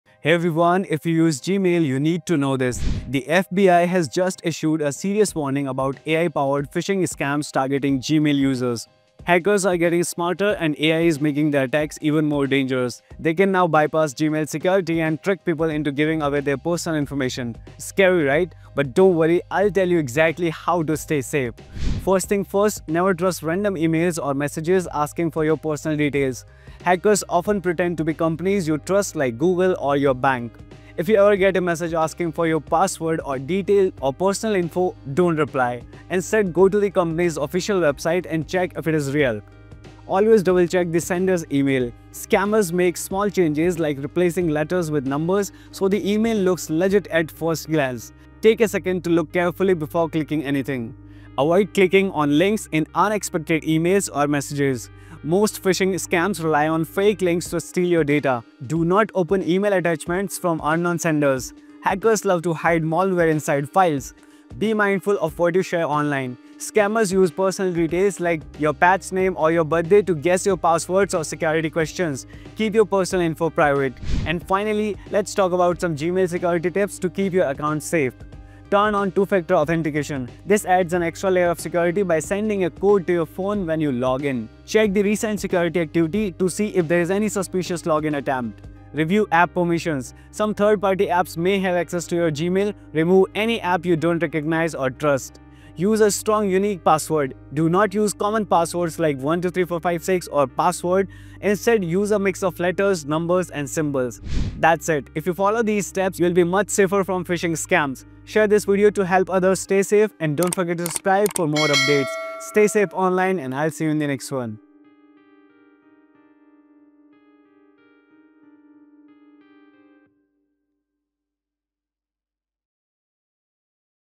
Gmail AI Scam Alert: Stay Safe with These Simple Steps
The FBI has issued a serious warning about AI-powered phishing scams targeting Gmail users. Hackers are using advanced AI to bypass security and steal personal information. But don’t worry, here’s how you can stay safe.

How to Protect Yourself:
Tip 1: Don’t trust random emails – Hackers pretend to be companies like Google or your bank. Always verify before responding.
Tip 2: Double-check email senders – Scammers change small details in email addresses to trick you. Look closely before clicking.
Tip 4: Don’t open unknown attachments – Hackers use them to spread malware. If you weren’t expecting a file, don’t open it.
Tip 5: Limit personal info online – Scammers use small details to guess passwords and security answers. Keep your info private.

Gmail Security Tips:
1. Enable Two-Factor Authentication (2FA) – Adds an extra security layer.
2. Check Gmail security settings – Go to Google Account → Security → Recent Security Activity for suspicious logins.
3. Review app permissions – Remove untrusted third-party apps.
4. Use a strong password – Avoid common passwords and use a mix of letters, numbers, and symbols.

By following these steps, you can protect yourself from phishing attacks.